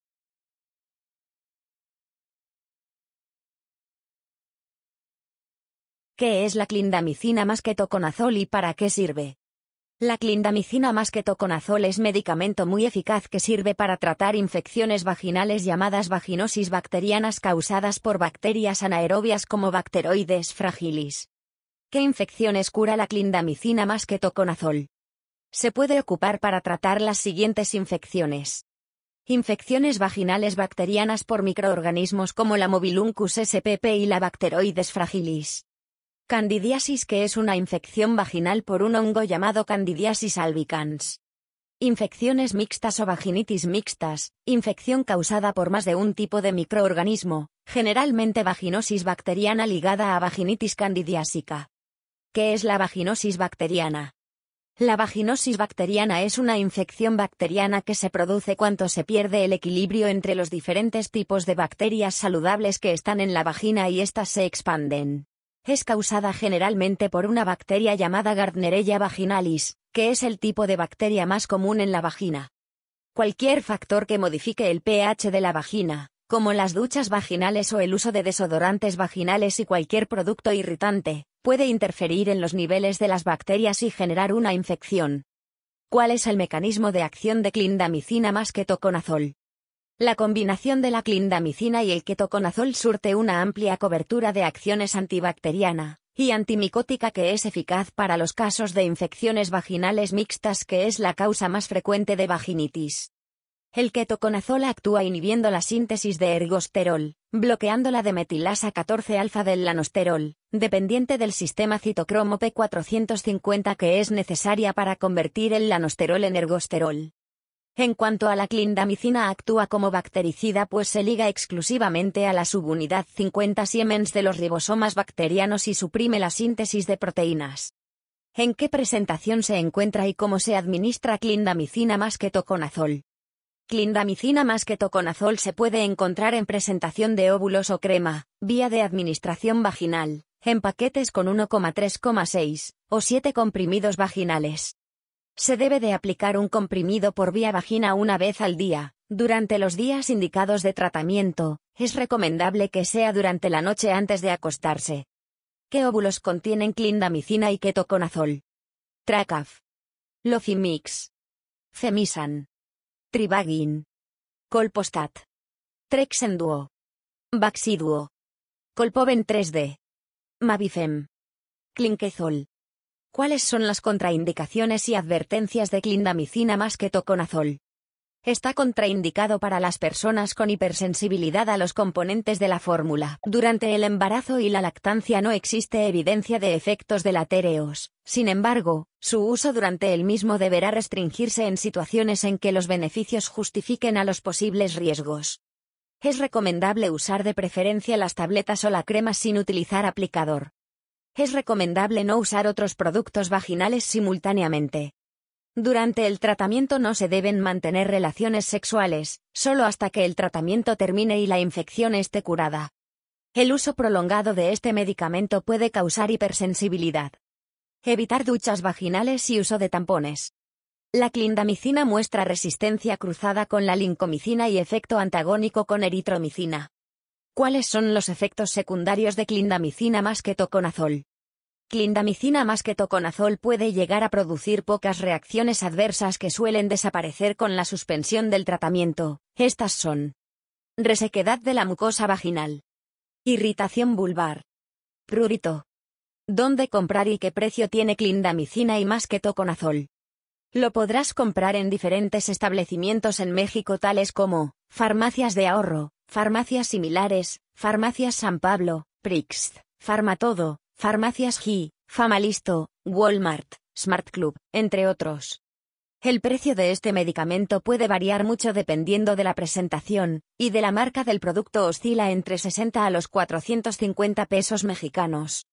CLINDAMICINA KETOCONAZOL que es y PARA QUE SIRVE, como usar clindamicina ketoconazol ovulos
¿Qué es la clindamicina más ketoconazol y para qué sirve?

La Clindamicina +  ketoconazol es  medicamento muy eficaz que sirve para tratar infecciones vaginales llamadas vaginosis bacterianas causadas por bacterias anaerobias como Bacteroides fragilis.

¿Qué infecciones cura la clindamicina + ketoconazol?

Se puede ocupar para tratar las siguientes infecciones:

Infecciones vaginales bacterianas por microorganismos como la Mobiluncus spp y la Bacteroides fragilis.

Candidiasis que es una infección vaginal por un hongo llamado Candidiasis Albicans.

Infecciones mixtas o vaginitis mixtas; infección causada por más de un tipo de microorganismo,  generalmente vaginosis bacteriana ligada a vaginitis candidiásica.

¿Qué es la vaginosis bacteriana?

La vaginosis bacteriana es una infección bacteriana que se produce cuanto se pierde el equilibrio entre los diferentes tipos de bacterias saludables que están en la vagina y estas se expanden.  Es causada generalmente por una bacteria llamada Gardnerella vaginalis,  que es el tipo de bacteria más común en la vagina.

Cualquier factor que modifique el PH de la vagina,  como las duchas vaginales o el uso de desodorantes vaginales y cualquier producto irritante,  puede interferir en los niveles de las bacterias y generar una infección.

¿Cuál es el mecanismo de acción de Clindamicina +  ketoconazol?

La combinación de la clindamicina y el ketoconazol surte una amplia cobertura de acciones antibacteriana y antimicótica que es eficaz para los casos de infecciones vaginales mixtas que es la causa más frecuente de vaginitis.

El Ketoconazol actúa inhibiendo la síntesis de ergosterol,   bloqueando la demetilasa 14-alfa del lanosterol,  dependiente del sistema citocromo P-450 que es necesaria para convertir el lanosterol en ergosterol.

En cuanto a la clindamicina actúa como bactericida pues se liga exclusivamente a la subunidad 50 S de los ribosomas bacterianos y suprime la síntesis de proteínas.

¿En qué presentación se encuentra y como se administra Clindamicina +  ketoconazol?

Clindamicina +  ketoconazol se puede encontrar en presentación de óvulos o crema,  vía de administración vaginal,  en paquetes con 1,3,6, o 7 comprimidos vaginales.

Se debe de aplicar un comprimido por vía vagina una vez al día,  durante los días indicados de tratamiento,  es recomendable que sea durante la noche antes de acostarse.

¿Qué óvulos contienen clindamicina y ketoconazol?

Tracaf.
Loffymix.
Femisan.
tribagyn.
Colpostat.
Trexen Duo.
vaxiduo.
Colpoben 3D.
Mavifem.
Clinkezol.

¿Cuáles son las contraindicaciones y advertencias de Clindamicina +  ketoconazol?

Está contraindicado para las personas con hipersensibilidad a los componentes de la fórmula.

Durante el embarazo y la lactancia no existe evidencia de efectos delatéreos,  sin embargo,  su uso durante el mismo deberá restringirse en situaciones en que los beneficios justifiquen a los posibles riesgos.
Es recomendable usar de preferencia las tabletas o la crema sin utilizar aplicador.
Es recomendable no usar otros productos vaginales simultáneamente.
Durante el tratamiento no se deben mantener relaciones sexuales,  solo hasta que el tratamiento termine y la infección este curada.
El uso prolongado de este medicamento puede causar hipersensibilidad.
Evitar duchas vaginales y uso de tampones.
La clindamicina muestra resistencia cruzada con la lincomicina y efecto antagónico con eritromicina.

¿Cuáles son los efectos secundarios de Clindamicina +  ketoconazol?

Clindamicina +  ketoconazol puede llegar a producir pocas reacciones adversas que suelen desaparecer con la suspensión del tratamiento,  estas son:

Resequedad de la mucosa vaginal.
Irritación vulvar.
Prurito.

¿Dónde comprar y que precio tiene clindamicina y + ketoconazol?

El precio de este medicamento puede variar mucho dependiendo de la presentación y de la marca del producto oscila entre 60 a los 450 pesos mexicanos.